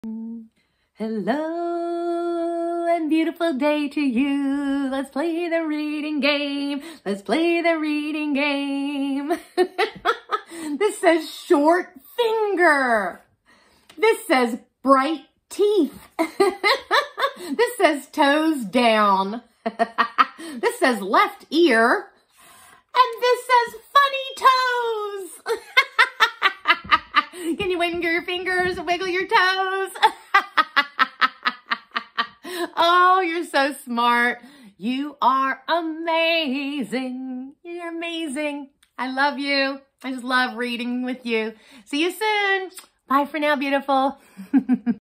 Lesson 60 F baby/preschooler learning to read English program.
Only couplets for these five days and then….we launch into phrases! Every day getting closer to sentences, and then a book! Keep showing up for your child, showing them these short videos sprinkled throughout the day. Begin with lesson 15 A, if you are just starting this program and build from there with the daily practice for your baby. You will be amazed to see them read in a matter of months. It is so cool! Aim to limit their viewing to these six videos a day. Try to be consistent, showing these videos to your child every day, and only when they are feeling happy.
10 couplets retired every five days and 10 new couplets introduced!